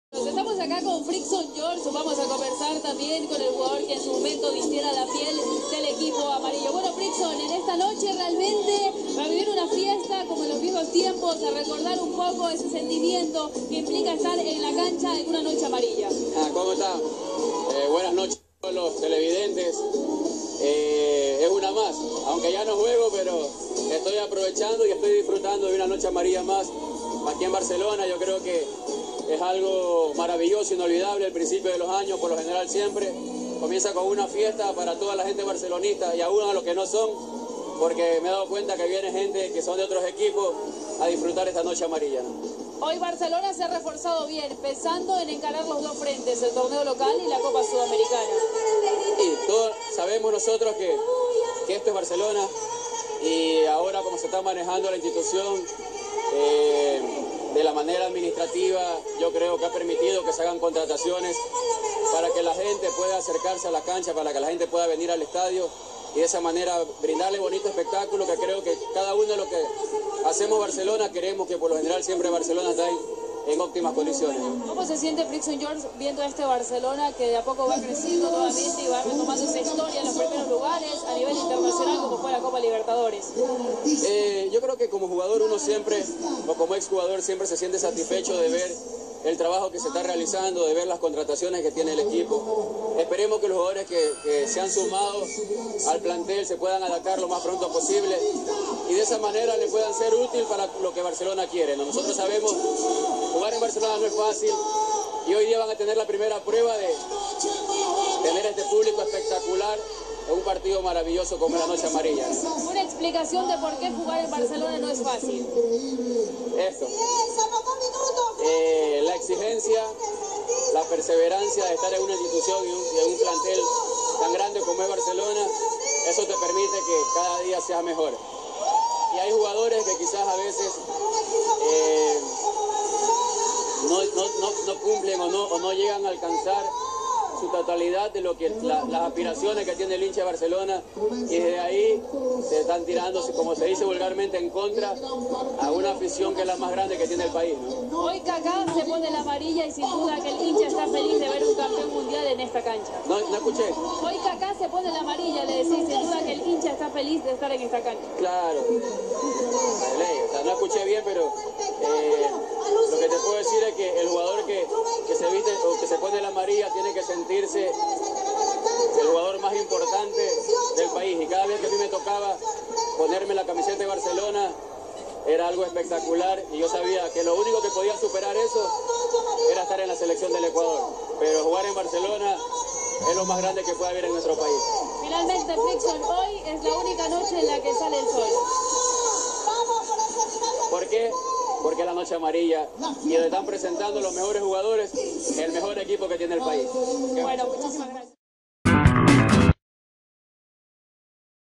ENTREVISTA A FRICSON GEORGE EN LA NOCHE AMARILLA 2018
Entrevista realizada a Fricson George por DIRECTV Sports, en la previa de la noche amarilla 2018 de Barcelona SC.